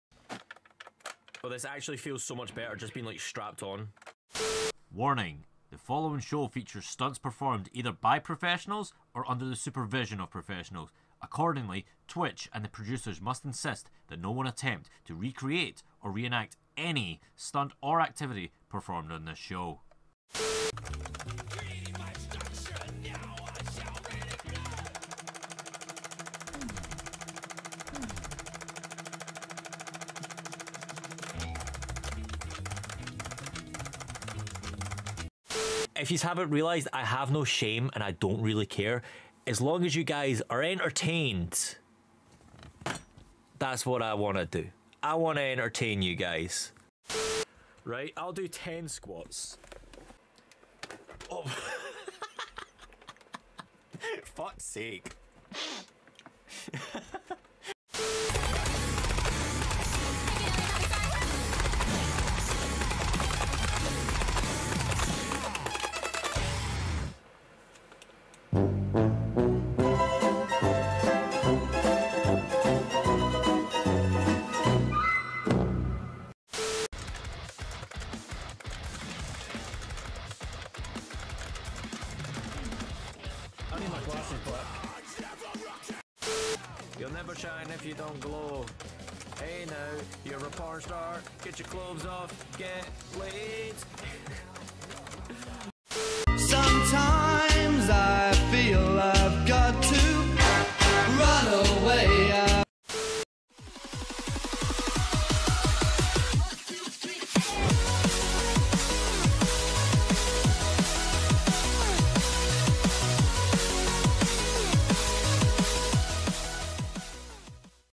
Best Twitch Clips of July 2021
Here we have some of the best of my Twitch clips from the month of July.